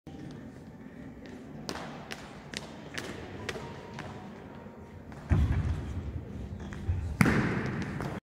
Толкание ядра - 29.01.2021 (12:41:19)
Чемпионат и первенства Санкт-Петербурга по легкоатлетическим многоборьям за Зимнем стадионе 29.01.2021.